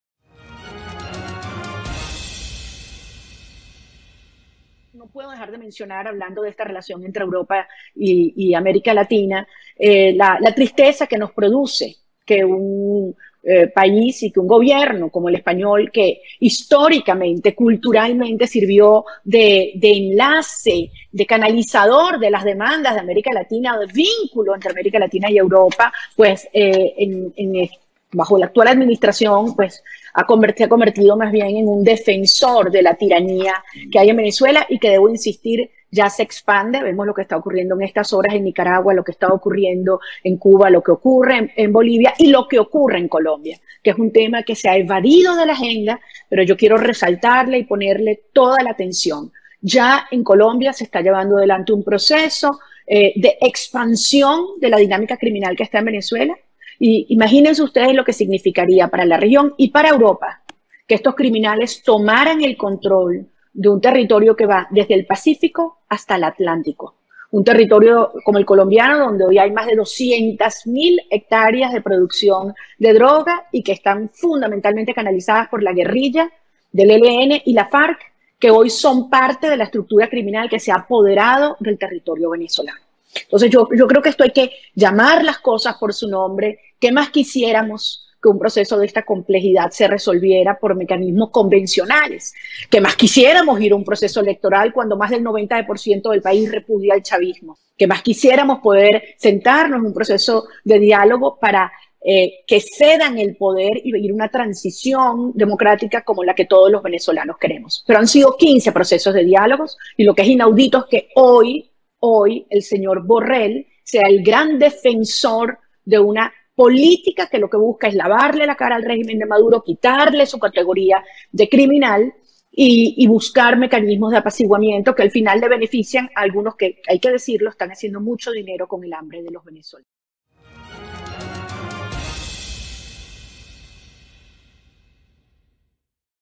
FIRMES Y CONTUNDENTES DECLARACIONES MARIA CORINA MACHADO A COMUNIDAD INTERNACIONAL
"Han Sido 15 procesos de diálogo y es inaudito que Borrell sea el gran defensor de una política para lavarle la cara al régimen de Maduro"